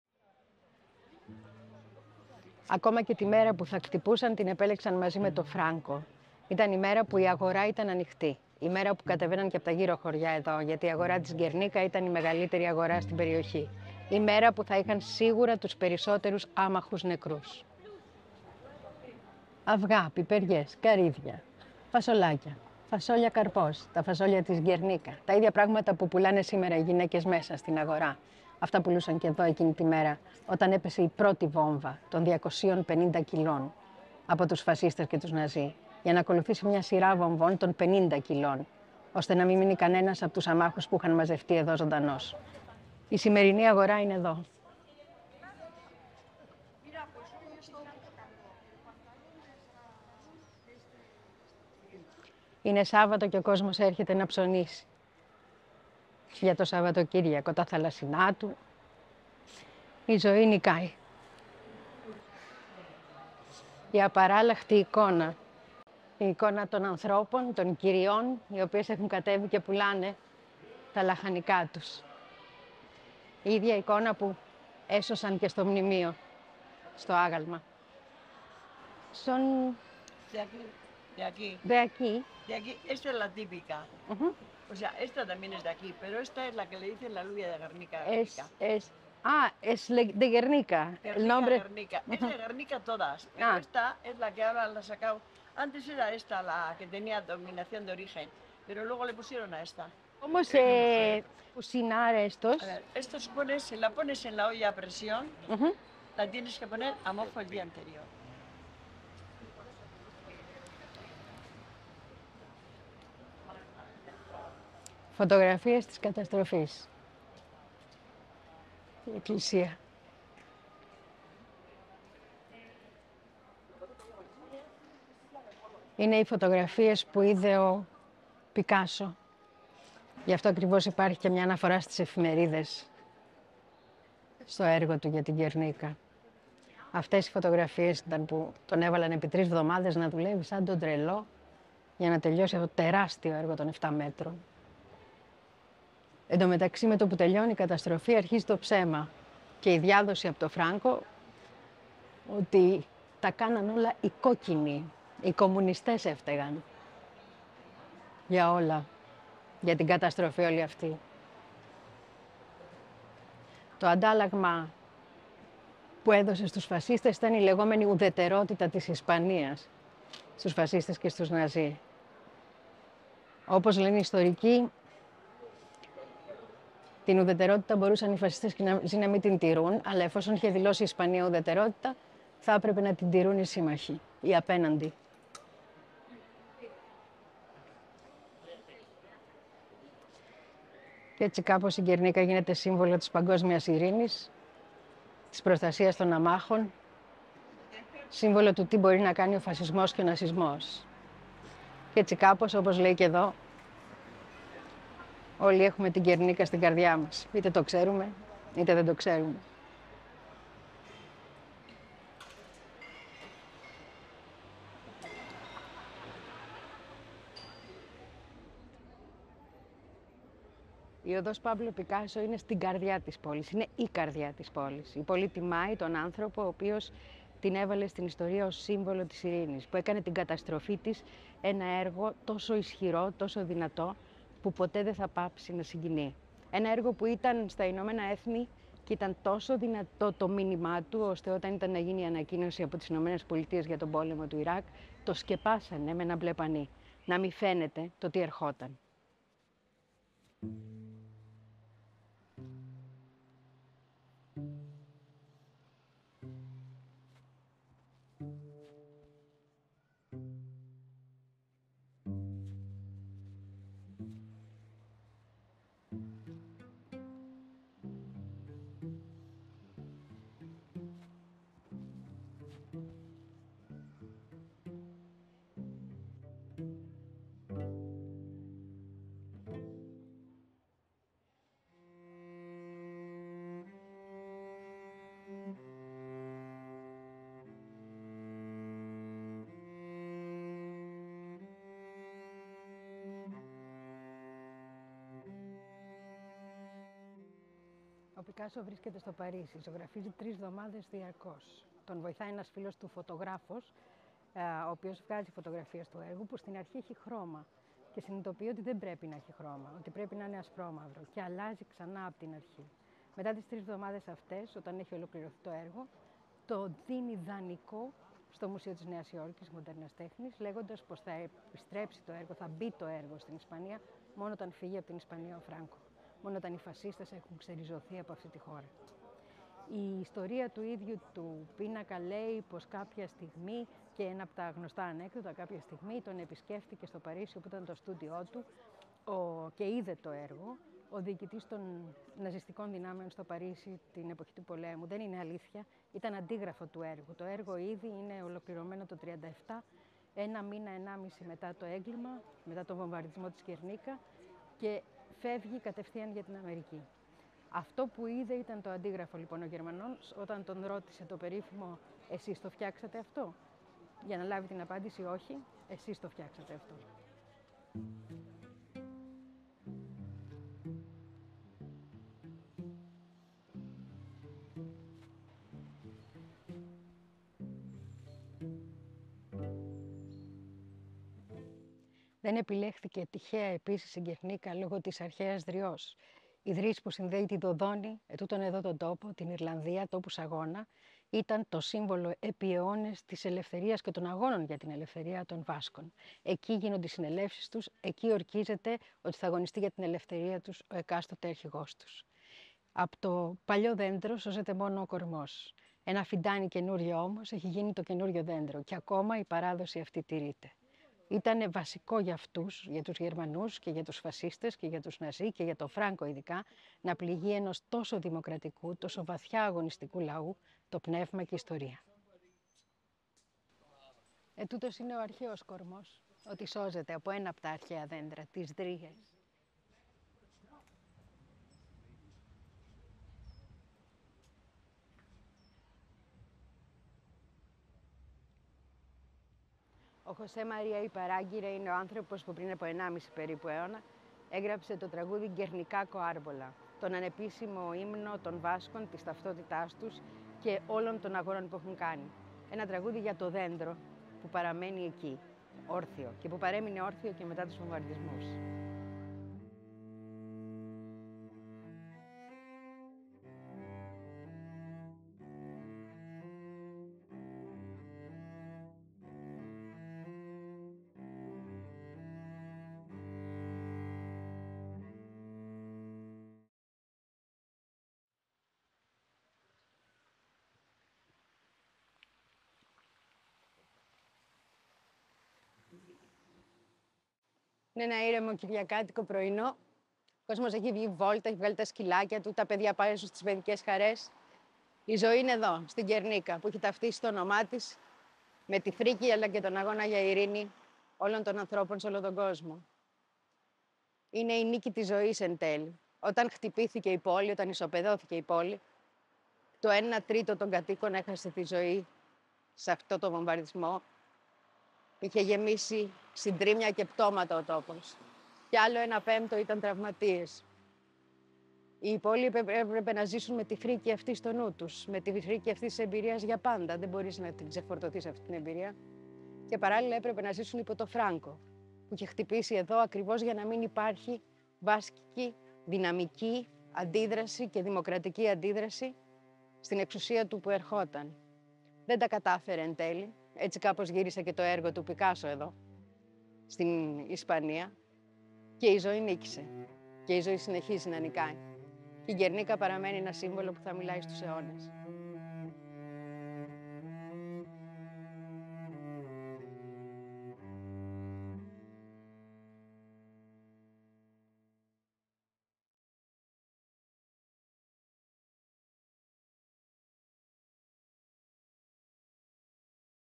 Γκερνίκα
Στις 26 Απριλίου 1937, οι γερμανοί ναζί και οι ιταλοί φασίστες βομβαρδίζουν την μικρή βασκική πόλη της Γκερνίκα (Guernica). Ο βομβαρδισμός γίνεται μετά από συμφωνία με τον φασίστα Φράνκο που υπόσχεται “ουδετερότητα της Ισπανίας” αν νικήσει στον εμφύλιο. Ο βομβαρδισμός δολοφονεί το ένα τρίτο των πέντε χιλιάδων κατοίκων, και τραυματίζει περί το ένα πέμπτο. Επίσης, καταστρέφει το 80% της πόλης, αλλά, κατά παραγγελία του Φρανκο, όχι το εργαστάσιο όπλων: ήταν ένας βομβαρδισμός κατά των αμάχων συγκεκριμένα. Στόχος να καμφθεί το αγωνιστικό πνεύμα των Βάσκων, με ένα πλήγμα στην πόλη που έχει ταυτιστεί με τους αγώνες τους και τον όρκο στην ελευθερία που αποτελεί «η Δρυς της Γκερνίκα». Η Λαμπρινή Θωμά και ο Νίκος Βεντούρας βρέθηκαν εκεί.